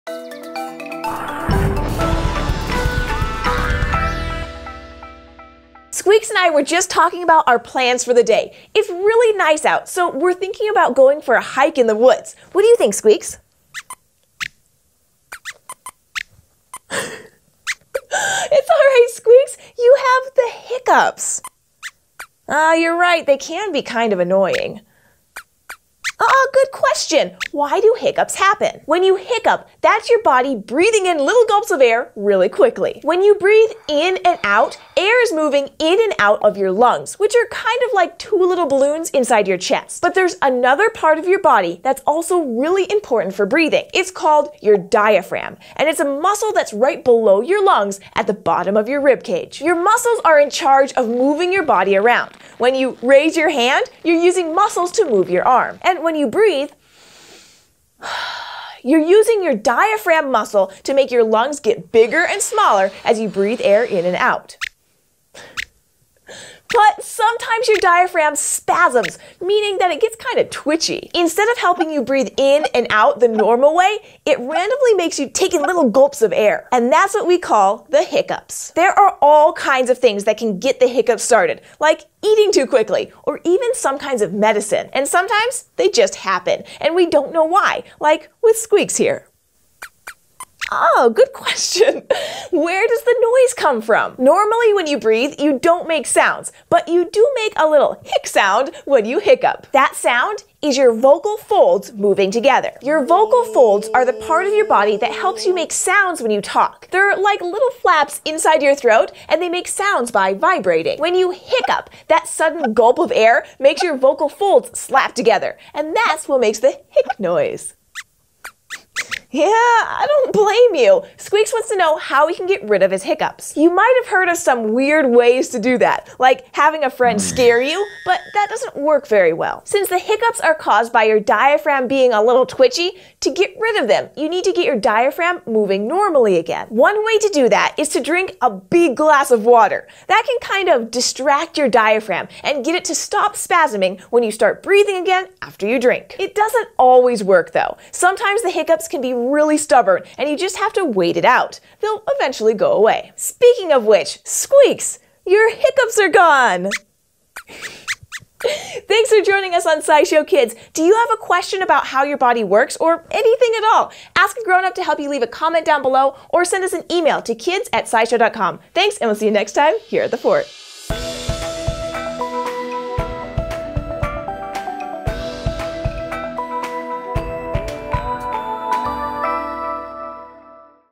Why Do We Get Hiccups? | Body Science for Kids
Hiccups can be really annoying and hard to get rid of, but do you know why they happen? Find out the answer and learn more about how your lungs work at the same time on today's SciShow Kids!